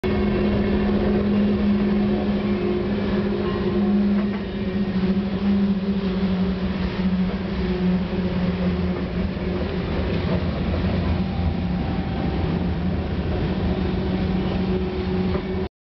Stampede Pass
Bathurst rides along side a Burlington Northern Train heading east over Stampede Pass.